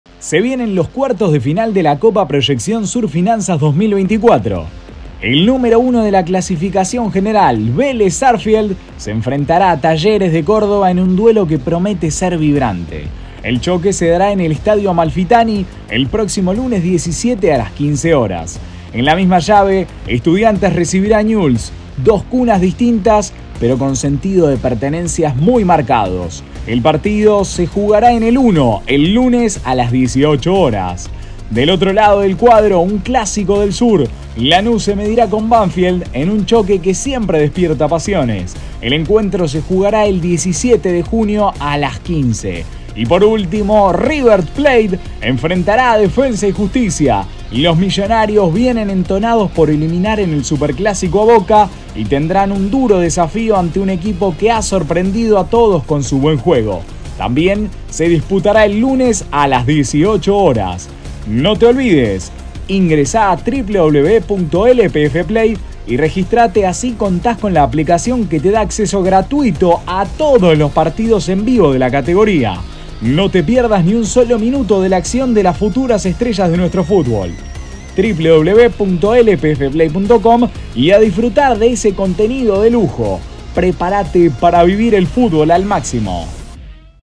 ¡Cuartos de final de la CopaProyección Sur Finanzas!
Se juegan los cuartos de la competencia, y vas a poder vivir todos los partidos en vivo y gratis